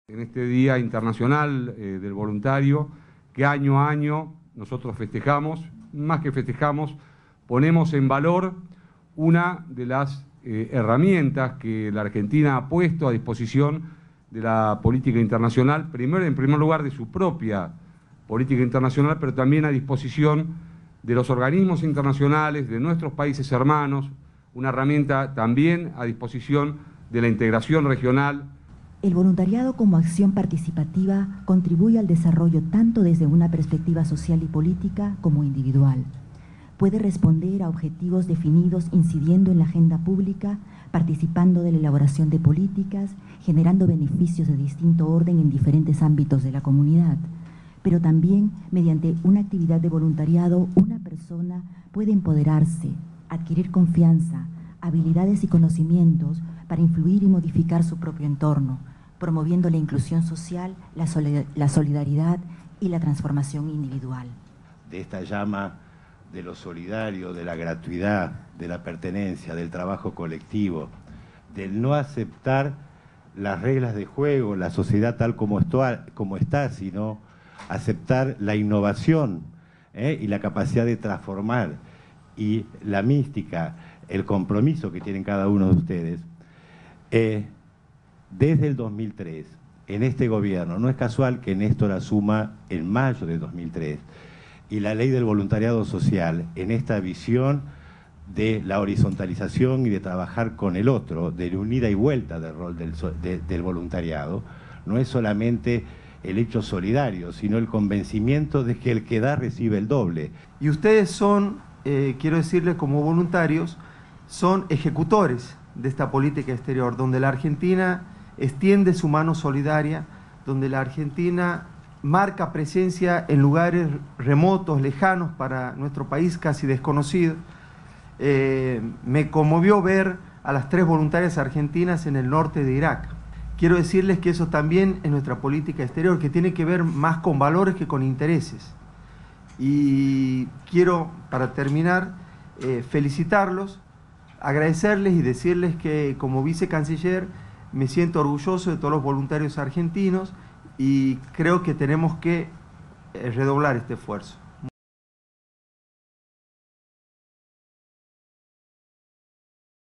Se realizó un homenaje a los voluntarios internacionales
La Cancillería, a través de la Comisión de Cascos Blancos, rindió un homenaje y distinguió a los voluntarios internacionales que este año participaron de las misiones de paz y ayuda humanitaria a Irak, Siria y Haití, entre otras.
El acto se llevó a cabo en el Palacio San Martín, en el marco del Día Internacional de los Voluntarios, que se conmemora cada 5 de diciembre, desde 1985, definido por la Asamblea General de las Naciones Unidas para impulsar medidas que permitan una mayor conciencia internacional sobre la contribución que voluntarias y voluntarios hacen en beneficio del desarrollo social en todo el mundo.
Del acto participaron el vicecanciller argentino, Eduardo Zuaín, el presidente de la Comisión Cascos Blancos, la representante del Programa de las Naciones Unidas para el Desarrollo (PNUD), Cecilia Ugaz, el secretario de Gestión y Articulación Institucional del Ministerio de Desarrollo Social, Aldo Marconetto y el vicecanciller de la Argentina, Eduardo Zuain.